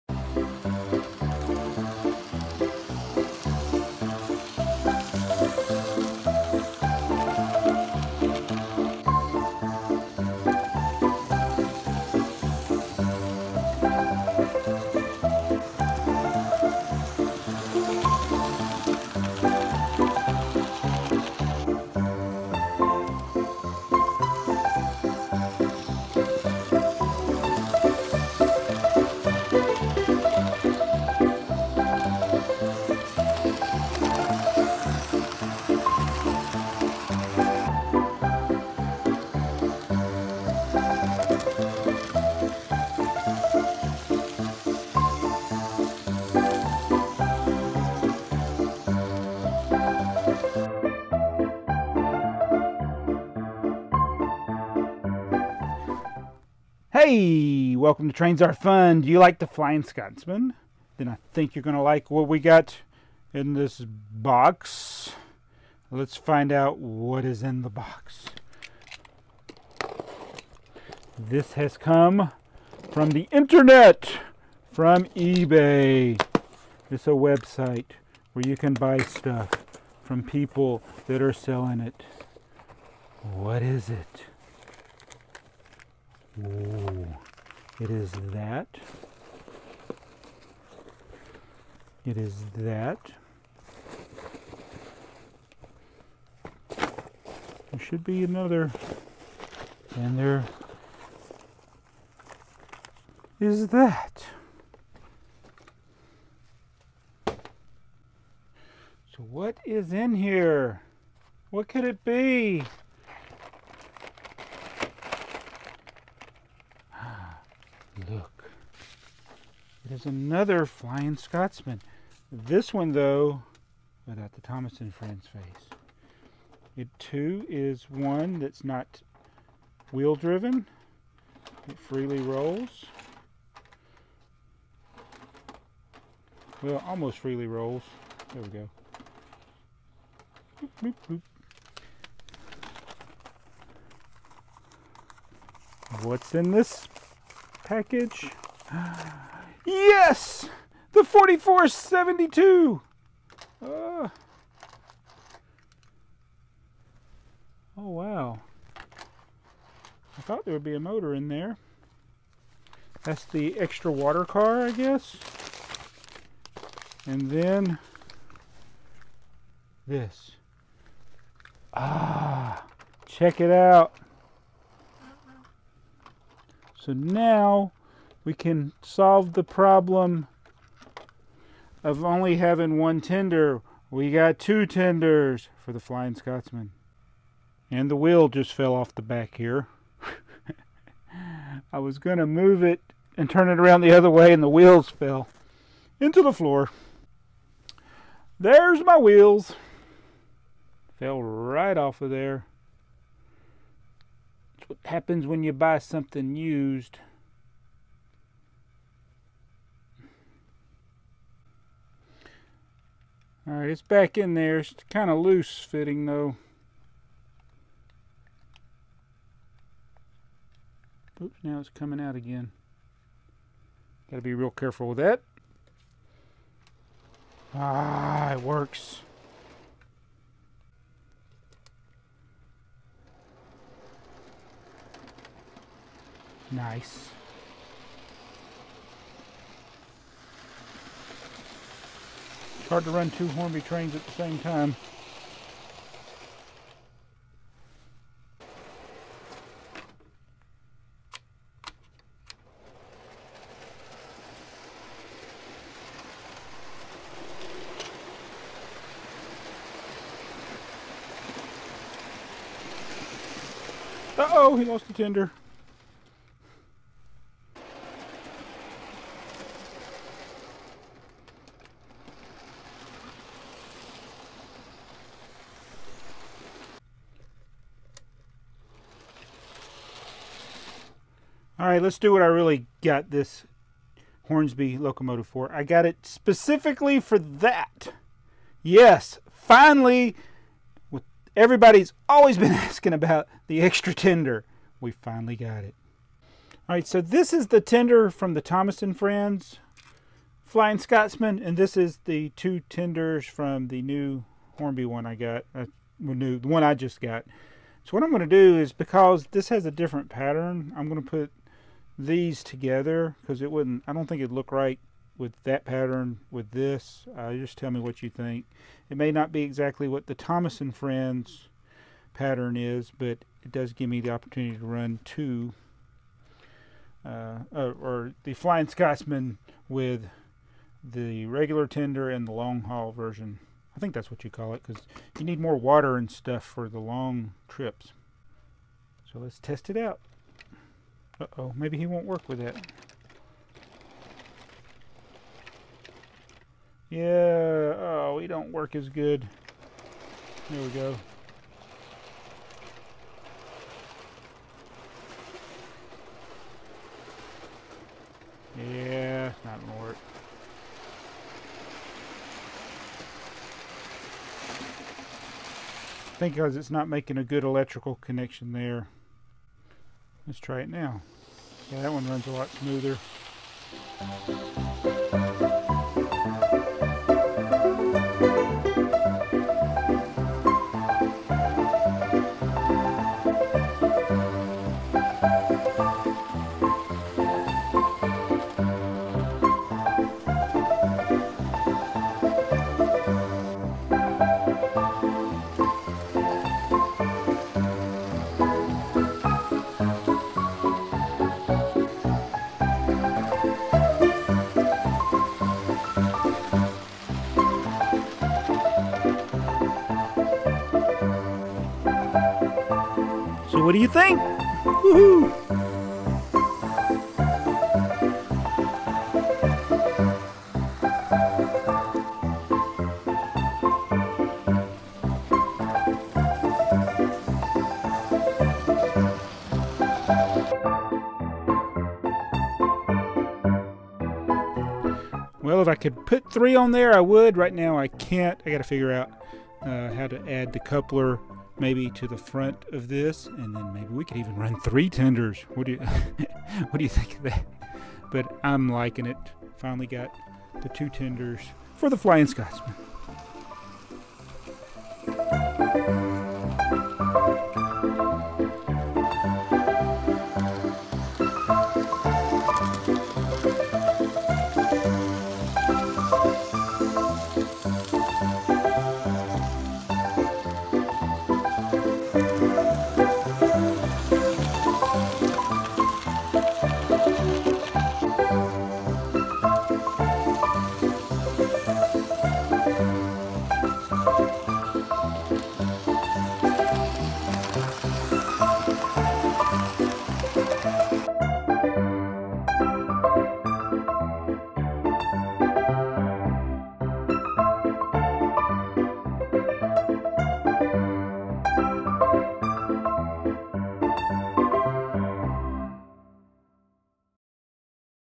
HORNBY FLYING SCOTSMAN THOMAS & FRIENDS 2 Tenders! OO/HO Scale Trains
Finally we got the extra tender for The Flying Scotsman! Found a cheap eBay lot from a US seller. Is not an exact match for the Thomas the Tank Engine version but it works good enough.

Kid and family friendly videos about toy trains, real trains, and more!
Thomas the Tank Engine, Chuggington, LEGO trains, and more fun!

LEGO on Amazon